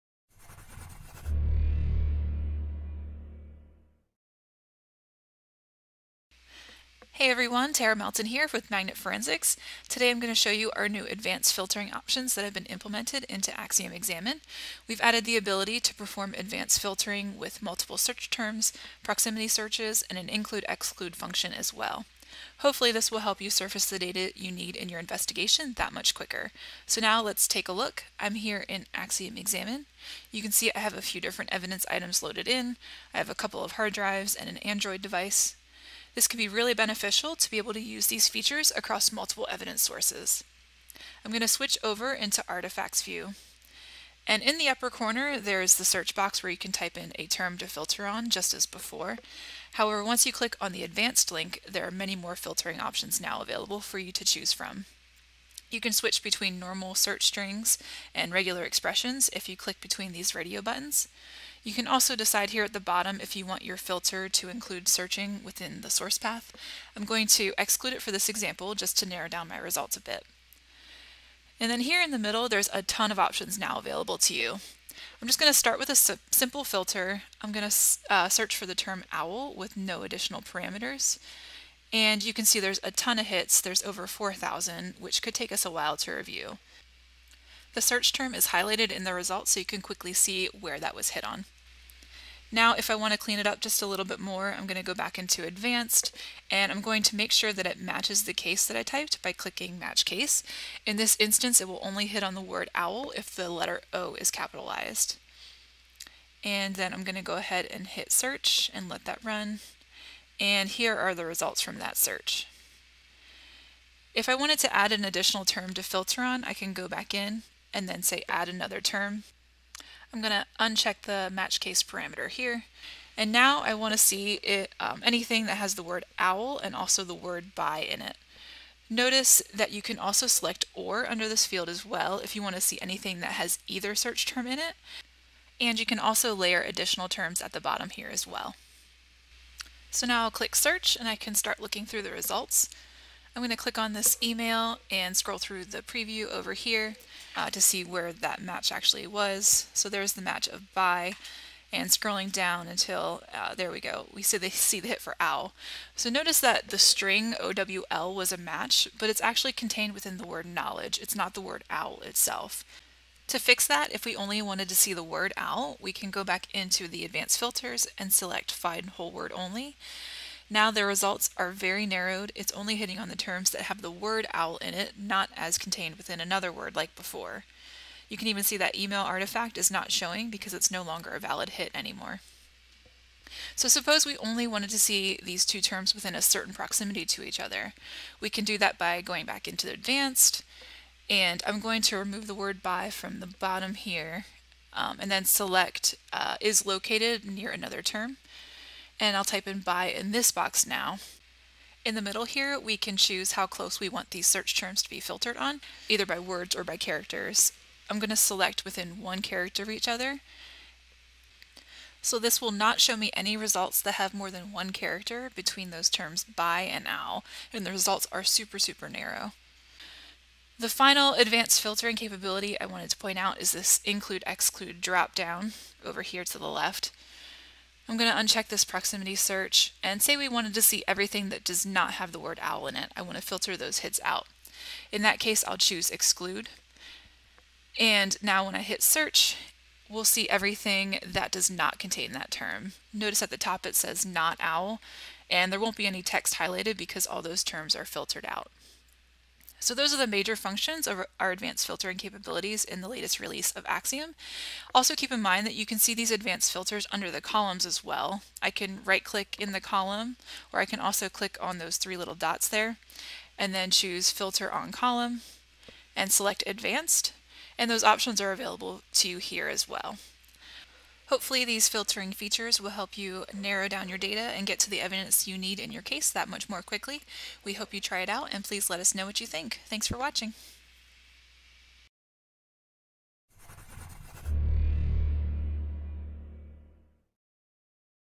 Advanced Filtering Options in Magnet AXIOM 3.6
With Magnet AXIOM 3.6, we've introduced the ability to perform advanced filtering with multiple search terms, proximity searches and include/exclude function, helping you surface the data you need in your investigations that much quicker.